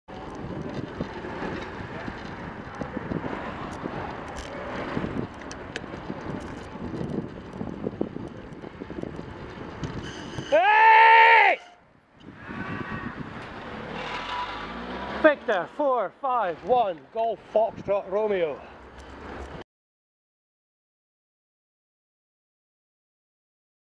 V451GFR right hook - blind driver?
The driver did not appear to be aware of my presence until I shouted. Although you can't see it on the video the shout made her jump and appears to have woken her up enough for her to start some avoiding action and braking. Without the shout she would probably have driven into me after I stopped. Can't see a bicycle directly in front of her car? Should have gone to specsavers, for the safety of the children in the car if not her own.

Sunday 24th August 2014 15:11